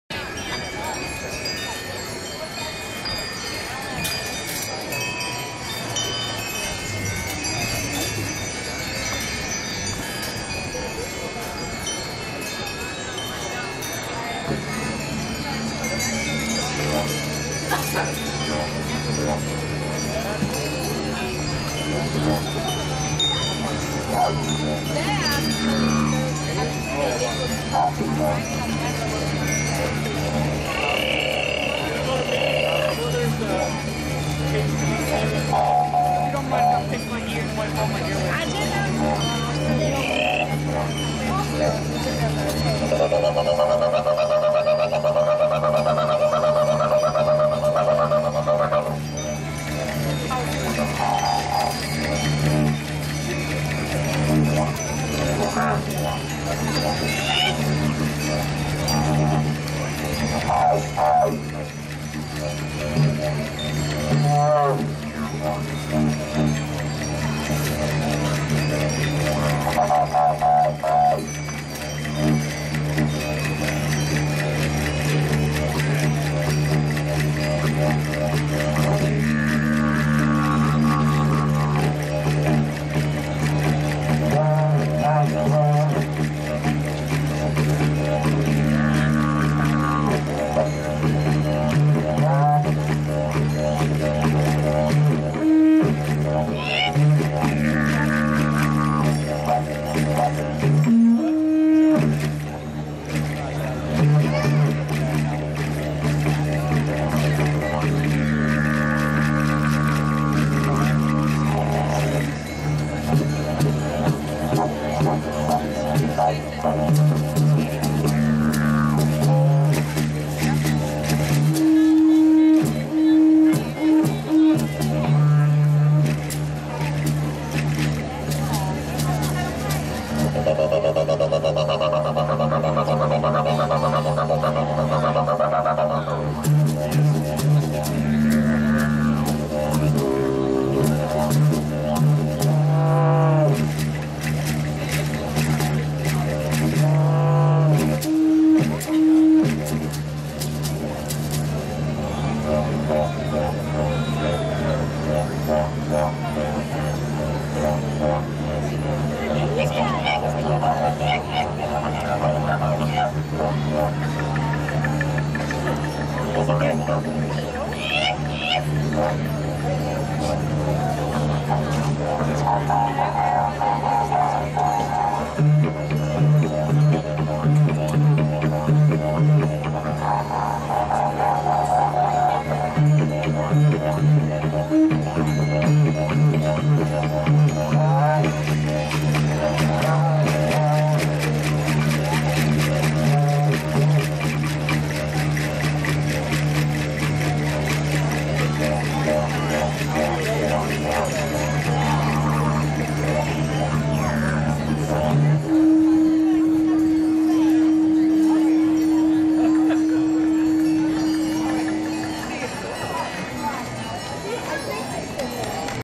Didgeridoo Virtuoso Rob Thomas of Inlakesh
Close your eyes, clear your mind, and let yourself float away into a world of sound.  The wind-blown chimes of Harmony Hollow Bells provide a back drop for another great and very trance-y performance by Rob Thomas of Inlakesh at Ann Arbor Art Fair 2010!

- they also have CDs!